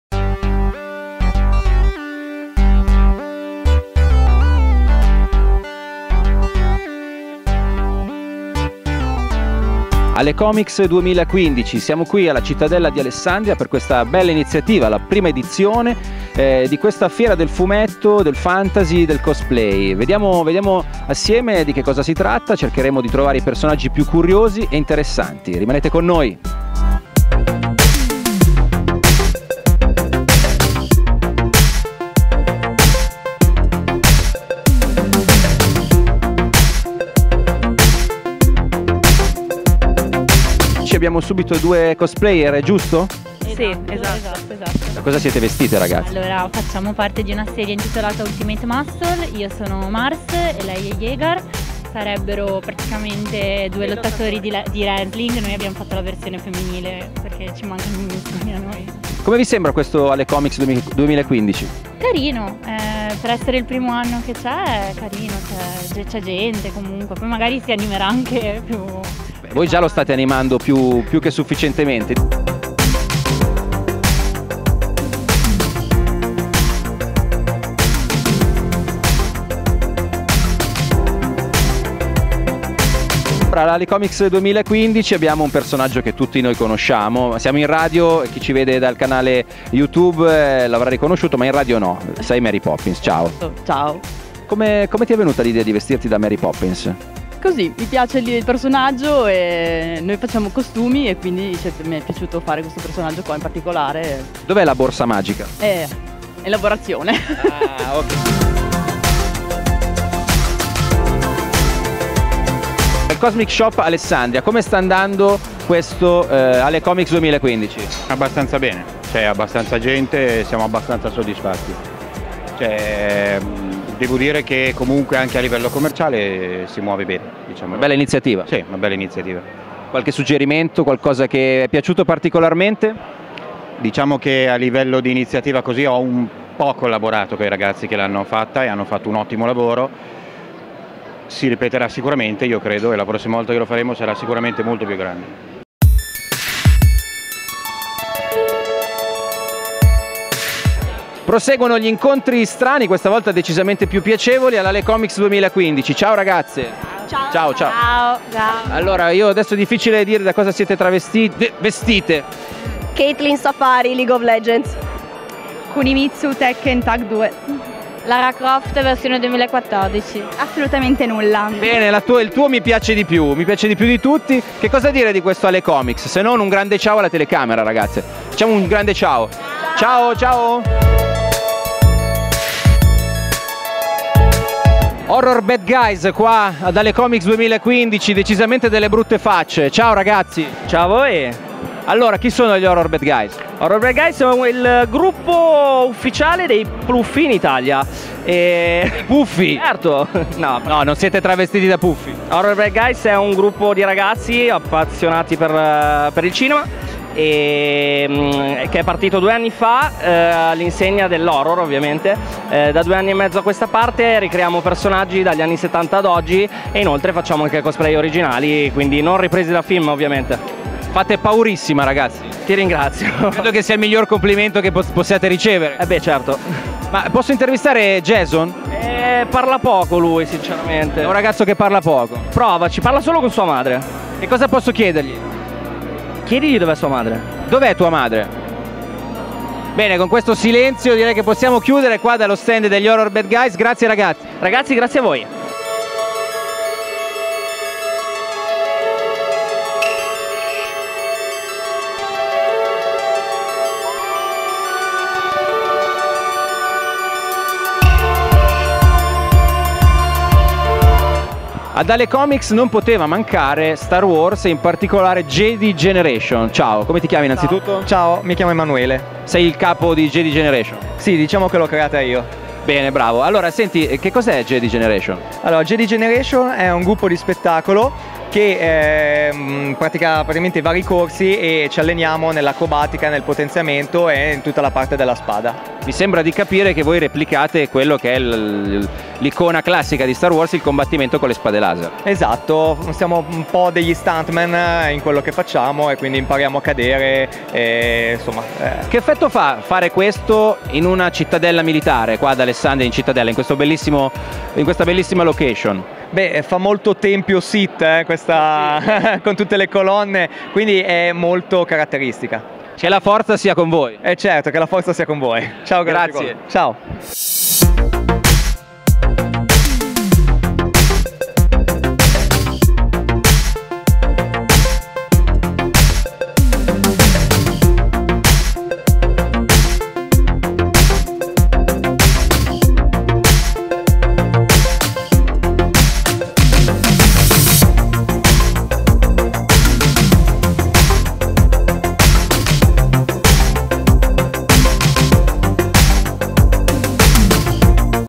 Alecomics 2015@Cittadella di Alessandria
L'Alecomics 2015 di Alessandria con cospalyer da tutta italia,la Jedi Generation,gli Horror Bad Guys,Giorgia Cosplay e tanti altri in una due giorni davvero ricca e divertente.
Interviste di Alessandro Gavazza
riprese video di Andrea Solimani